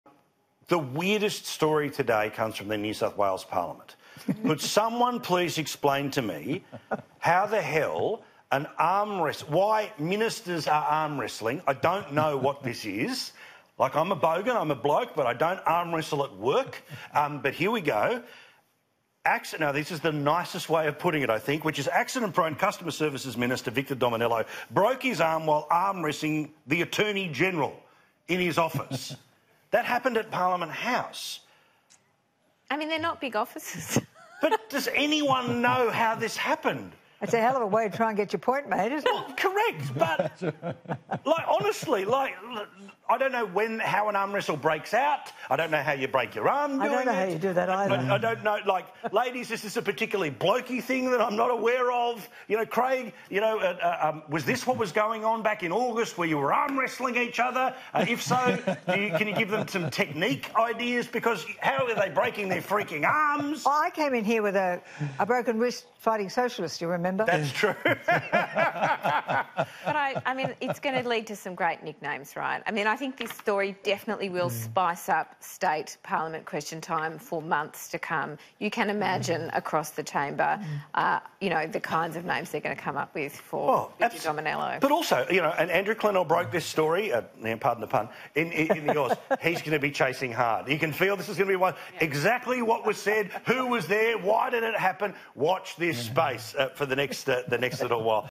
NSW Minister’s arm-wrestling mishap will ‘spice up’ parliament
New South Wales Customer Service Minister Victor Dominello has broken his arm while arm-wrestling Attorney-General Mark Speakman in his office.